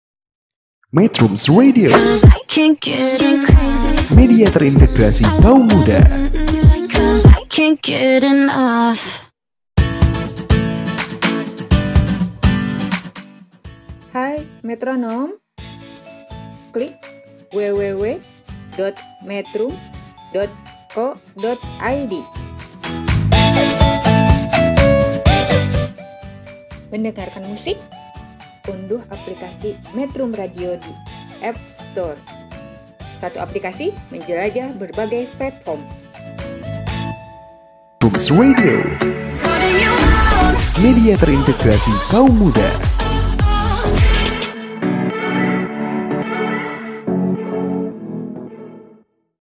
Membuat Video Animasi Metrum Radio Dengan KineMaster
Metrum Radio Channel Jelajah Komunitas

Ternyata membuat video animasi Metrum Radio dengan KineMaster itu tidak memerlukan komputer atau laptop, dengan gawai/ handphone saja kita bisa membuat animasi dengan bagus dan mudah

Follow juga akun Instagram kami di @metrum.co.id
Fanspage FB @metrum.co.id